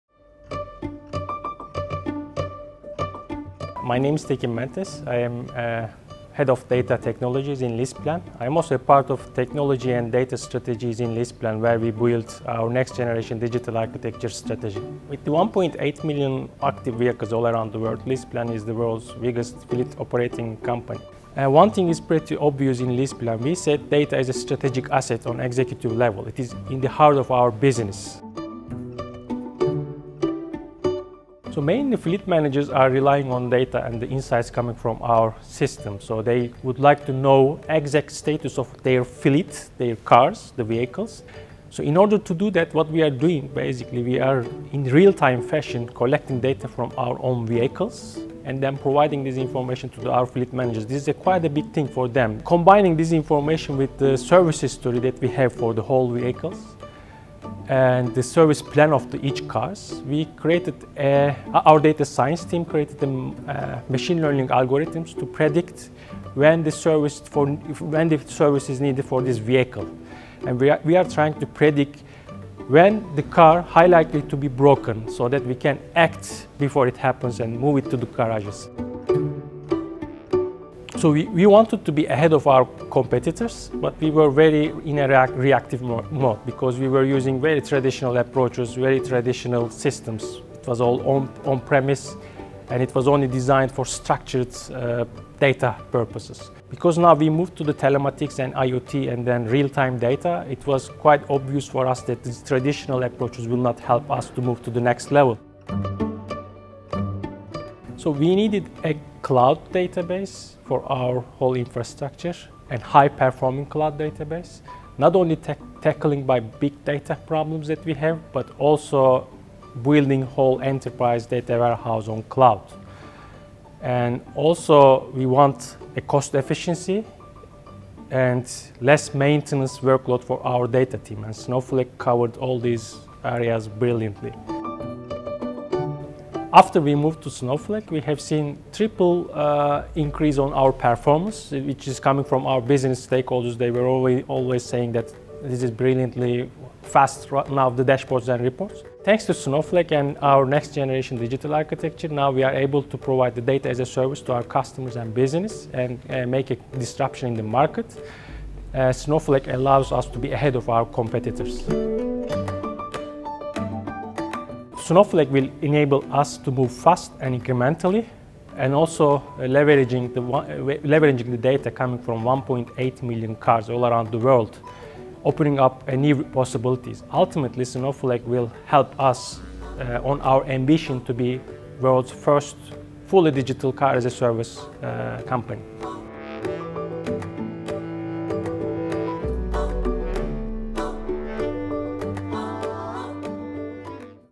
Predicting What Car Services Are Needed with Snowflake | Snowflake Inc.
Leaseplan, a fleet management company, uses data for a machine learning algorithm to predict when services are needed for vehicles ahead of when they break down.

With an on premise solution, Leaseplan could not analyze this data fast enough in real-time. Now, with Snowflake they have seen a 3X performance increase and can provide their data as a service to their customers and business, while staying ahead of their competitors.